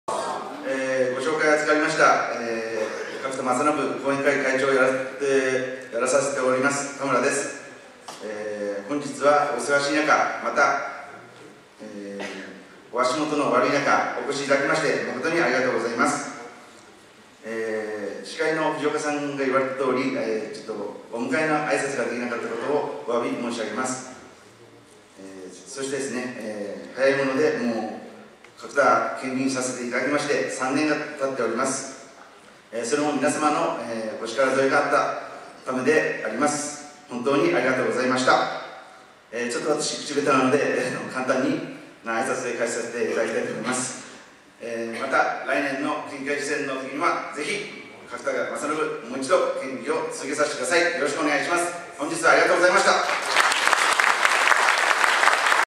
みんなの党 角田まさのぶ県議 新春の集い
栃木市、岩舟町選挙区選出の角田まさのぶ県議の新春の集いが、栃木市サンプラザにて、鈴木俊美栃木市長、渡辺喜美みんなの党代表、渡辺美知太郎参議院議員、渡辺幸子県議、鶴貝大祐県議、岡賢治栃木市議、天谷浩明栃木市議、小松英夫鹿沼市議、舘野裕昭鹿沼市議、荒木英知宇都宮市議ら、支持者約450人が集い、新春を祝うと共に、さらなる団結を誓った。